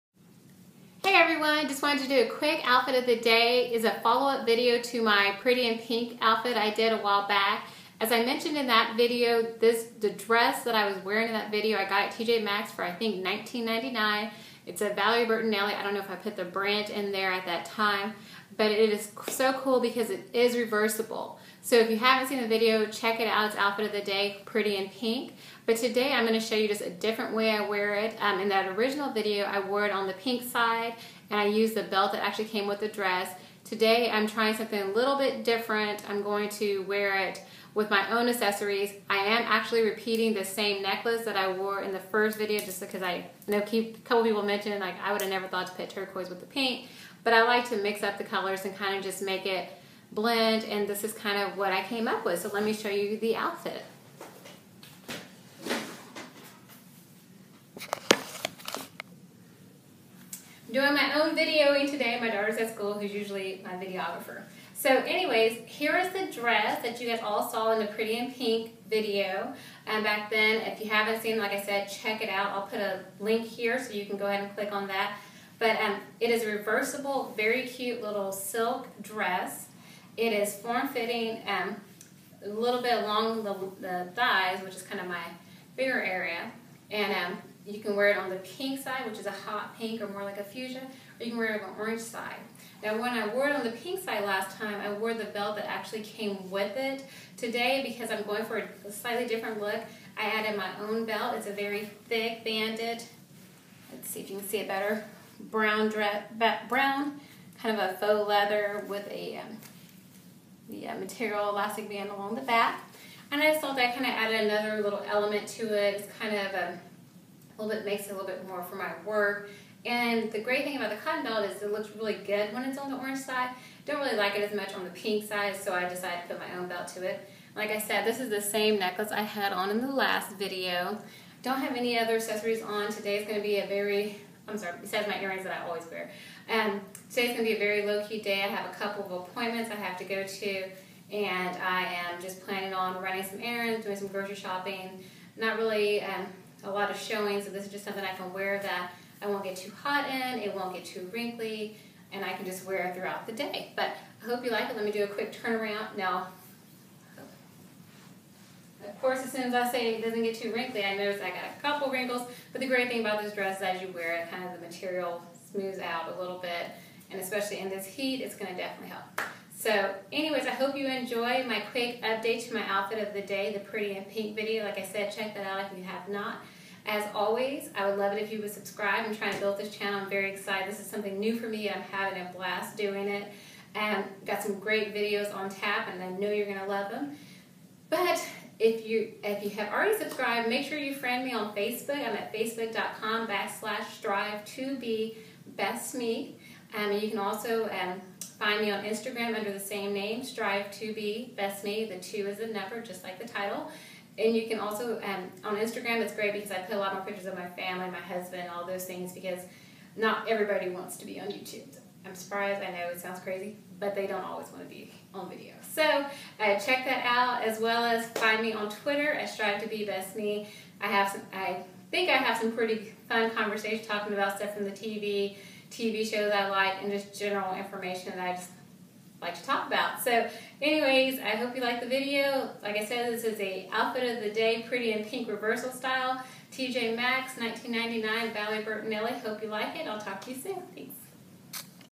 STUNNING IN ORANGE~ OUTFIT OF THE DAY~
Check out the PRETTY IN PINK dress repurposed and now a whole new look with new accessories and a brand new look. Great for the fall and simple and easy this dress was under $30!!

CHECK ME OUT ON INSTAGRAM: STRIVE2BEBESTME

THANK YOU FOR ALL YOUR SUPPORT I'M HAVING A BLAST MAKING THESE VIDEOS AND I HOPE YOU ENJOY THE TOPICS AND CONTENT!

THIS DRESS IS FROM TJ MAXX AND WAS UNDER $30 AND IS REVERSIBLE!! CHECK IT OUT ON THE FLIP SIDE AT

ALL VIDEOS ARE MY ORIGINAL CONTENT AND I OWN THE RIGHTS TO THE VIDEO IN THE ENTIRETY.